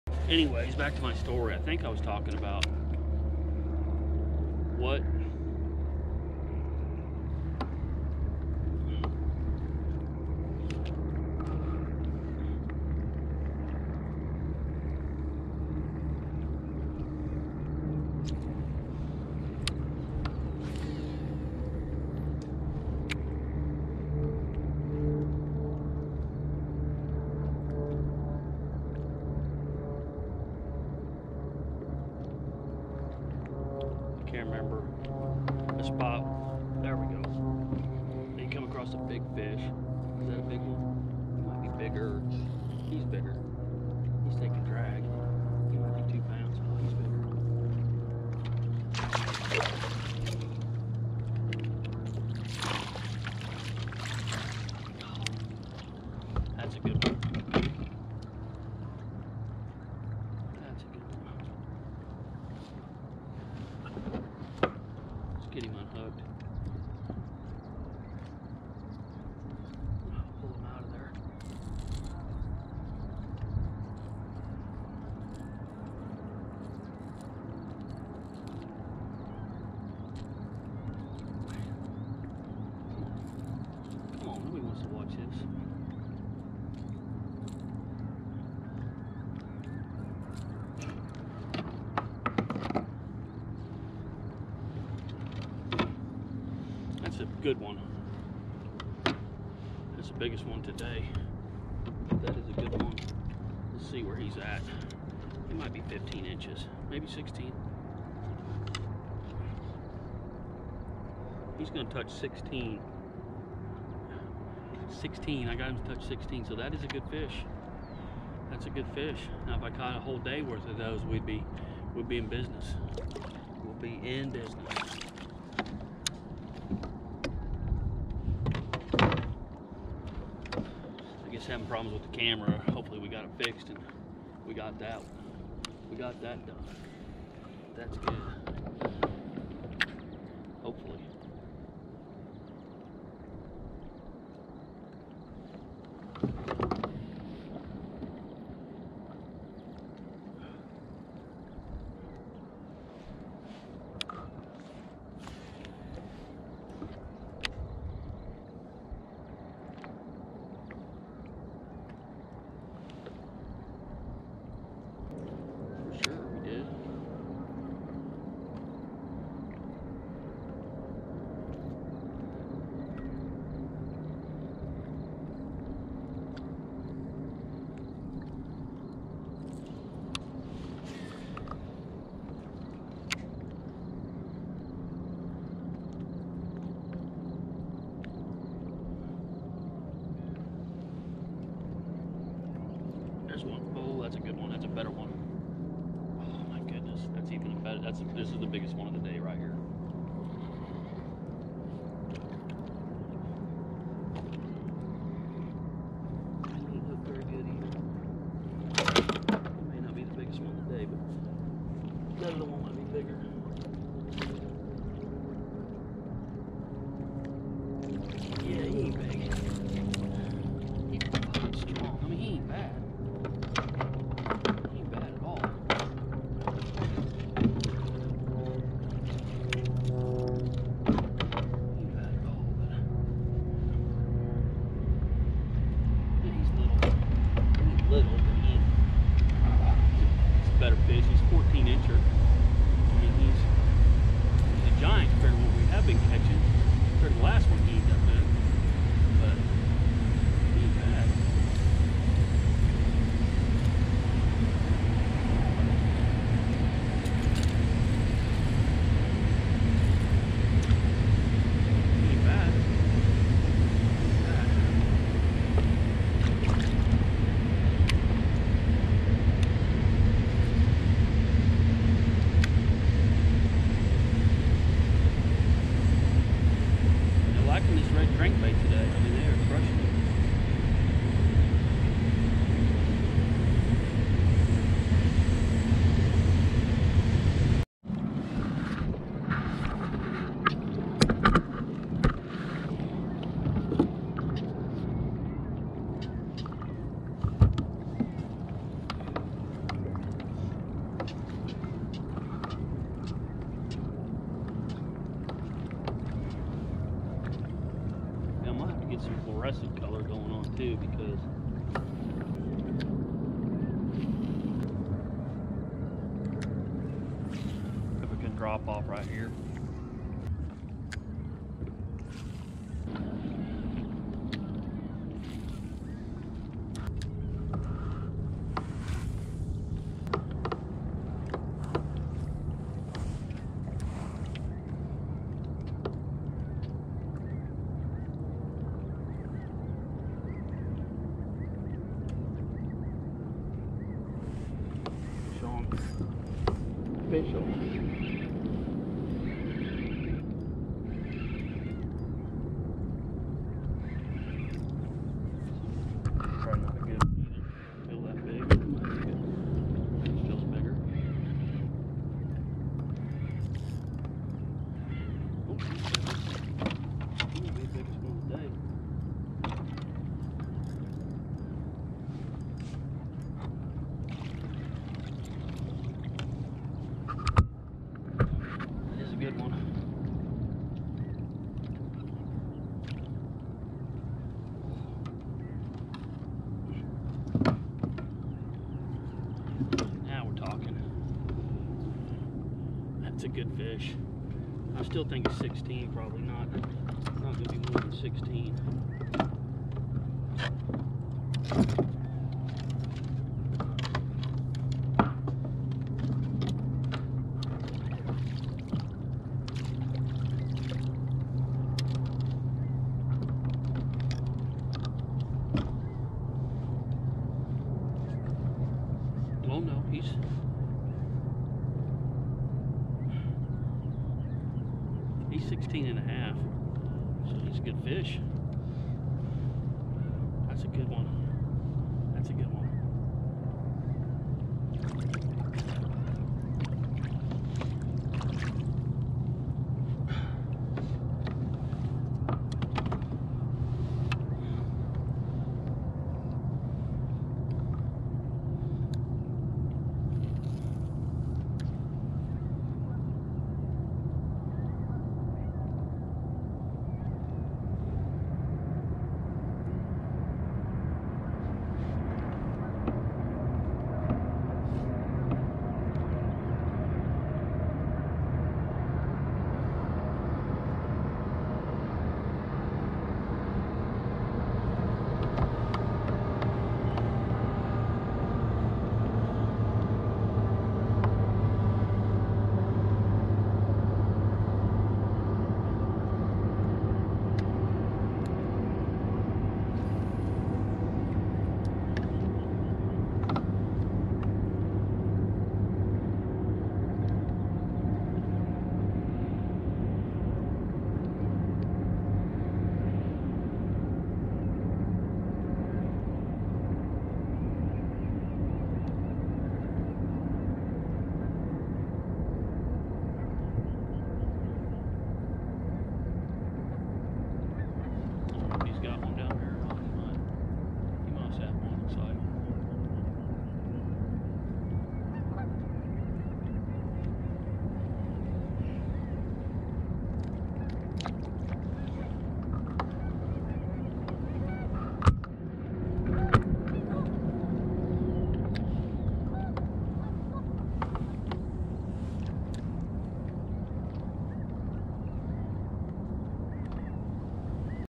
May 2022 Columbia River. Smallmouth Bass
Best Weather we have seen all year. I was Lucky enough to get on the water.
Smallie Bite is here for the summer!